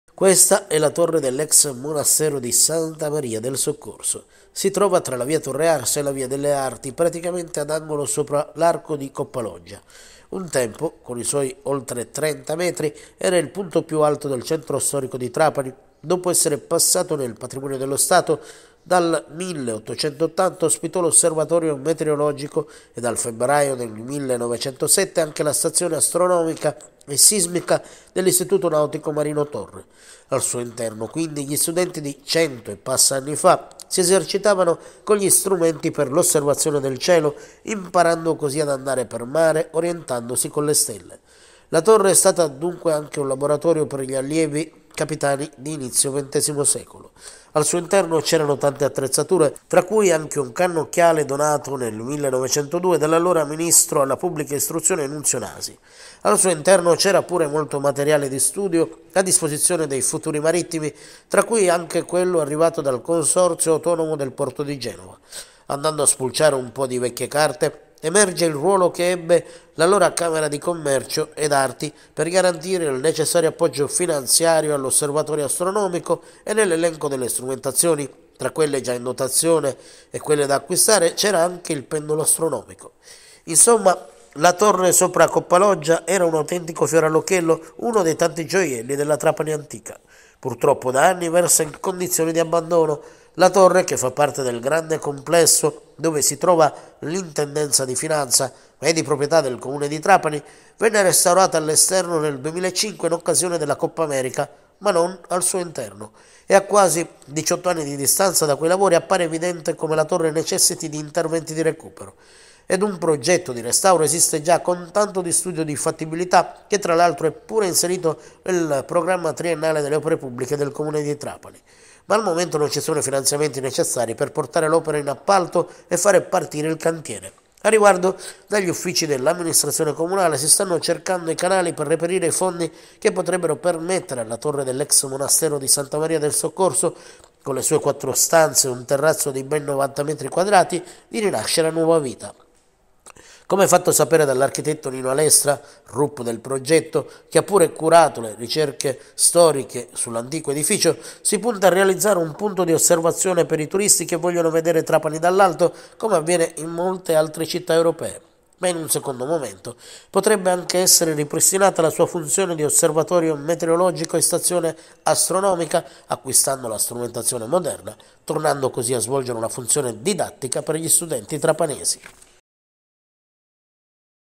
I tesori di Trapani: la Torre dell'ex monastero di Santa Maria del Soccorso
Questa è la Torre dell'ex monastero di Santa Maria del Soccorso. Si trova tra la via Torrearsa e la via delle Arti, praticamente ad angolo sopra l'arco di “Coppa Loggia”. Un tempo, con i suoi oltre 30 metri, era il punto più alto del centro storico di Trapani: dopo essere passato nel patrimonio dello Stato, dal 1880 ospitò l'Osservatorio meteorologico e dal febbraio del 1907 anche la Stazione astronomica e sismica dell'istituto Nautico “Marino Torre”. Al suo interno, quindi, gli studenti di cento e passa anni fa si esercitavano con gli strumenti per l'osservazione del cielo, imparando così ad andare per mare orientandosi con le stelle.
La Torre è stata dunque anche un laboratorio per gli allievi capitani di inizio ventesimo secolo. Al suo interno c'erano tante attrezzature, tra cui anche un cannocchiale donato nel 1902 dall'allora ministro alla Pubblica istruzione Nunzio Nasi. Al suo interno c'era pure molto materiale di studio a disposizione dei futuri marittimi, tra cui anche quello arrivato dal Consorzio autonomo del porto di Genova. Andando a spulciare un po' di vecchie carte emerge il ruolo che ebbe l'allora Camera di Commercio ed Arti per garantire il necessario appoggio finanziario all'Osservatorio astronomico e nell'elenco delle strumentazioni, tra quelle già in dotazione e quelle da acquistare, c'era anche il pendolo astronomico.
Insomma, la Torre sopra "Coppa Loggia" era un autentico fiore all'occhiello, uno dei tanti gioielli della Trapani antica. Purtroppo da anni versa in condizioni di abbandono. La Torre, che fa parte del grande complesso dove si trova l'Intendenza di Finanza ma è di proprietà del Comune di Trapani, venne restaurata all'esterno nel 2005 in occasione della Coppa America, ma non al suo interno. E a quasi a 18 anni di distanza da quei lavori appare evidente come la Torre necessiti di interventi di recupero. Ed un progetto di restauro esiste già, con tanto di studio di fattibilità, che tra l'altro è pure inserito nel Programma triennale delle opere pubbliche del Comune di Trapani. Ma al momento non ci sono i finanziamenti necessari per portare l'opera in appalto e fare partire il cantiere.
A riguardo, dagli uffici dell'amministrazione comunale si stanno cercando i canali per reperire i fondi che potrebbero permettere alla Torre dell'ex monastero di Santa Maria del Soccorso, con le sue quattro stanze ed un terrazzo di ben 90 metri quadrati, di rinascere a nuova vita. Come fatto sapere dall'architetto Nino Alestra, Rup del progetto, che ha pure curato le ricerche storiche sull'antico edificio, si punta a realizzare un punto di osservazione per i turisti che vogliono vedere Trapani dall'alto come avviene in molte altre città europee. Ma in un secondo momento potrebbe anche essere ripristinata la sua funzione di Osservatorio meteorologico e Stazione astronomica acquistando la strumentazione moderna, tornando così a svolgere una funzione didattica per gli studenti trapanesi.
Mario Torrente